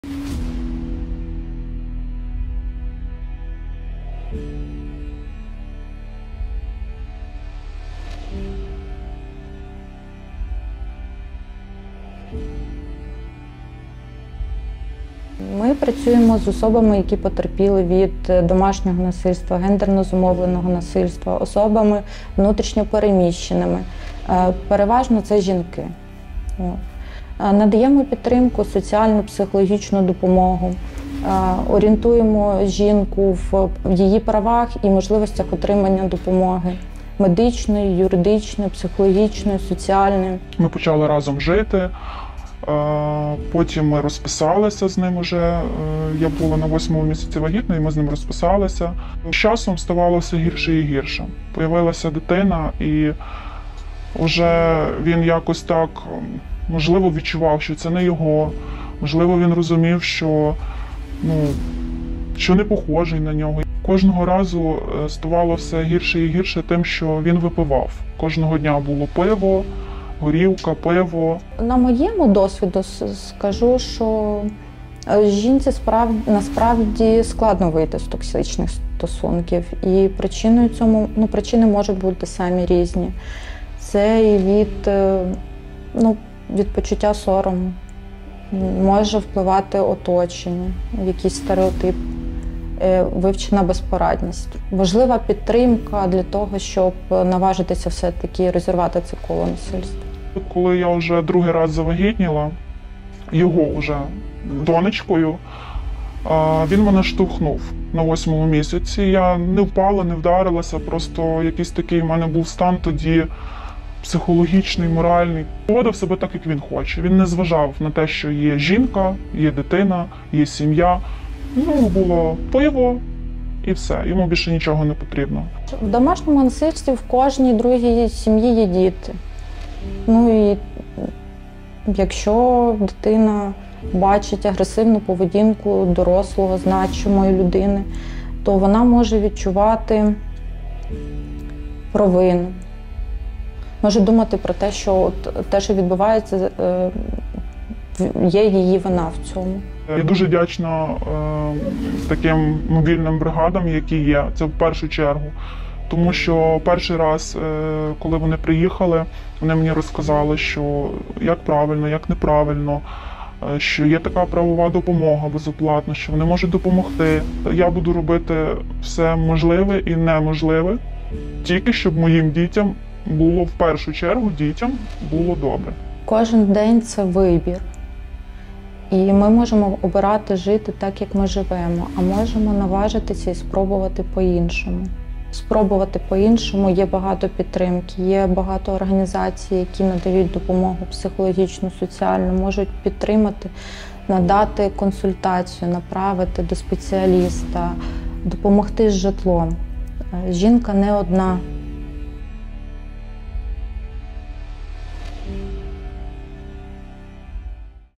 Як жінці РОЗІРВАТИ ТОКСИЧНІ стосунки: КУДИ звертатись по допомогу
Оксана (імʼя змінене) потерпала від психологічного насильства свого чоловіка. Він лаявся, зневажливо ставився до дітей і зловживав алкоголем.

Нещодавно жінка таки розірвала стосунки із кривдником. У цьому їй допомогла мобільна бригада соціально-психологічної допомоги UNFPA. Тамтешня психологиня каже: за час повномасштабної війни повідомлень про домашнє насильство стало менше. Але це не означає, що його немає – люди просто звертаються рідше.

Як розірвати токсичні стосунки, як працює мобільна бригада і куди звертатися за допомогою – дивіться у нашому відео.

Відео підготовлене в межах Програми з протидії та запобігання гендерно зумовленому насильству UNFPA, Фонду ООН у галузі народонаселення, за підтримки уряду США (Бюро USAID з питань гуманітарної допомоги).